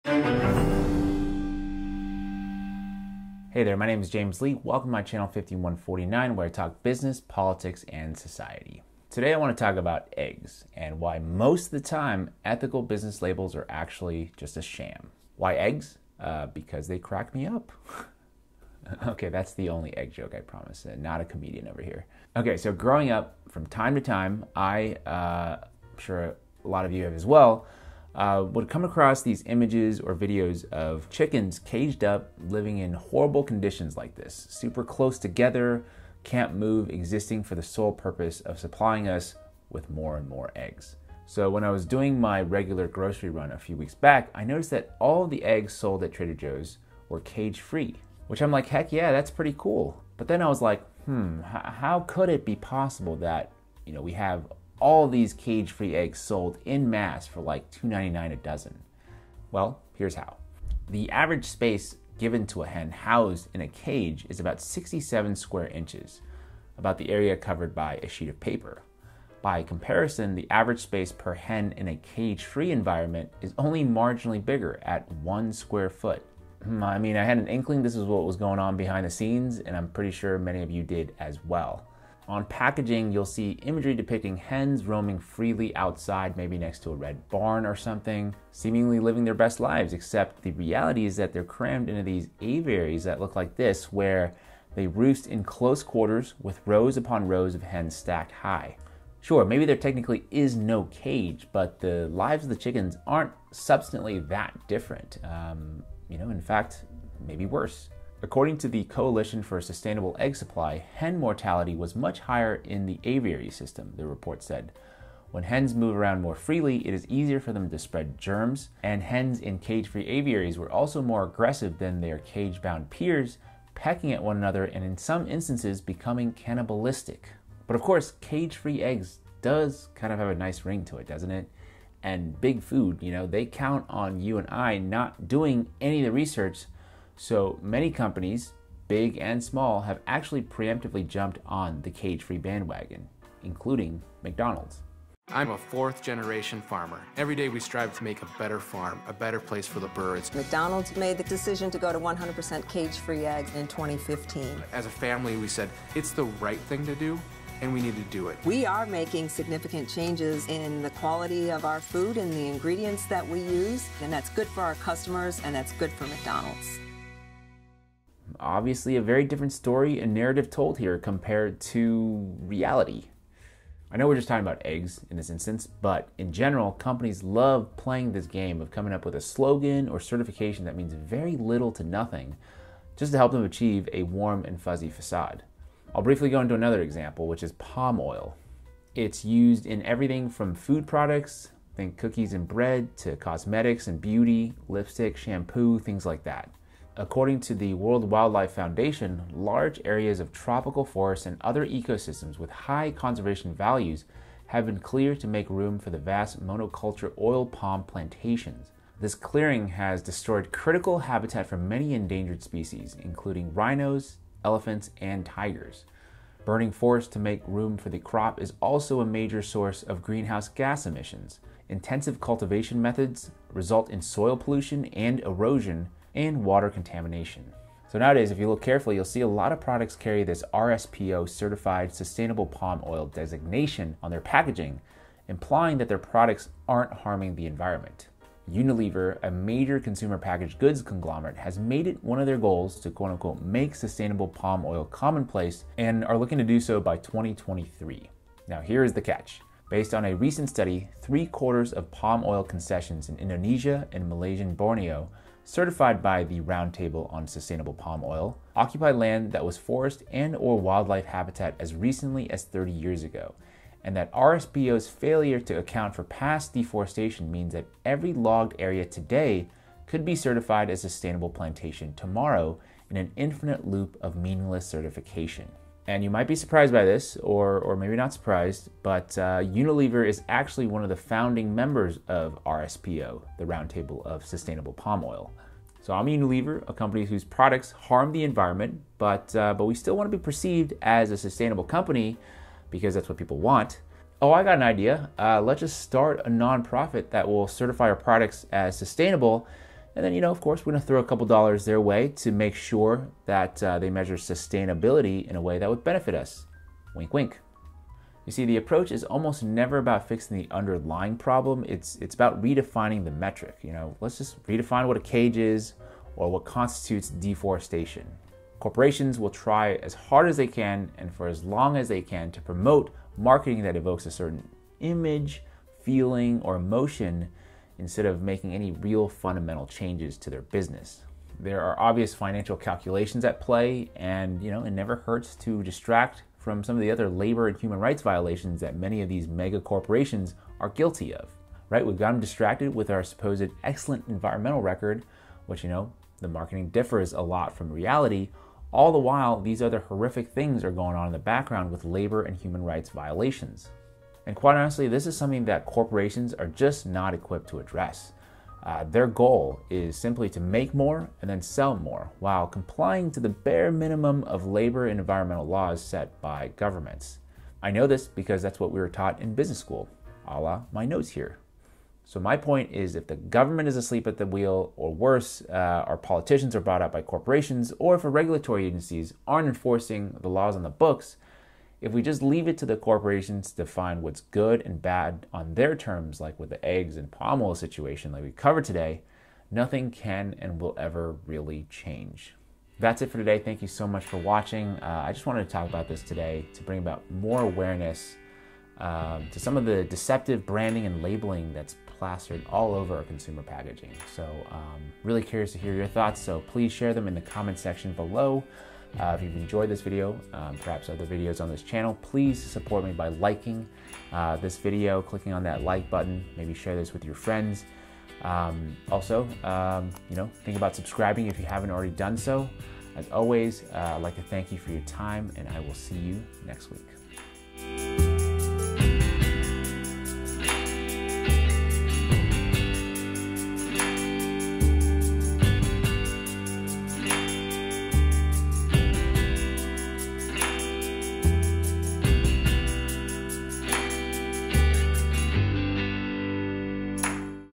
the LIES you're being told by "sustainable capitalism"
This week on 51/49, James, through a discussion of "cage free eggs" and "sustainable palm oil", reveals the great lengths to which a businesses will go, to project an aura of sustainability without actually being sustainable.

That's right--you might think you are helping the world by buying certain products, such as those labeled with an RSPO certification, but the truth is, at the end of the day, a sustainable business just doesn't make much business sense (aka "sustainable capitalism is just capitalism).

0:00 Introduction
0:23 The Myth Behind Cage Free Eggs
3:05 The Truth About Sustainable Business Labels
6:12 Conclusion

51/49 with James Li - Commentary on business, politics, and the other things (from an independent perspective)